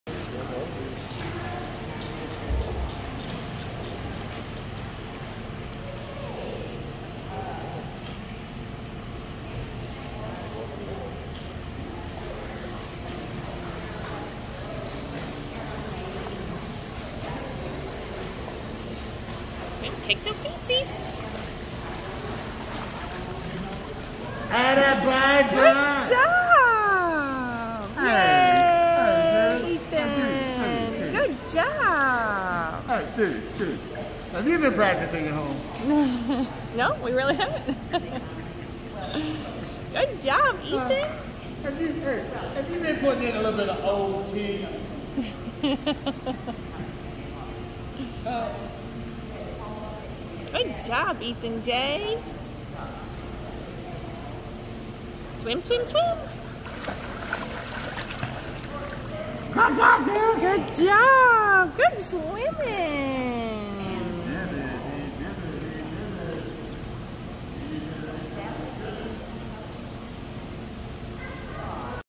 Ethan ISR swim, day 10, 1/29/21....vid 1
Ethans 10th lesson with ISR. So happy he is starting to have fun!